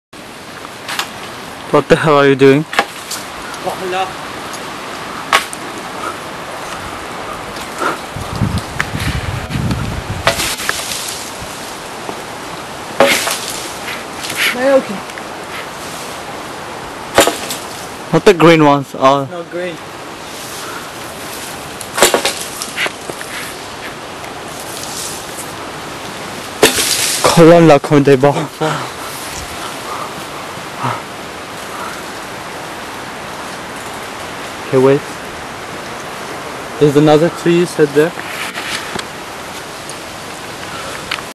Getting coconuts from trees in Fuerteventura
TRying to get coconuts from a tree in Fuerteventura